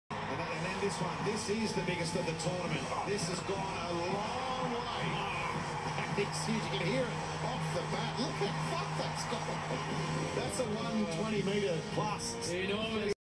Warnie drops F Bomb during BBL
Have a listen to some Shane Warne gold as he describe's Hilton Cartwright's brutul power-hitting against the Sydney Sixers at the MCG on Australia Day, 2021.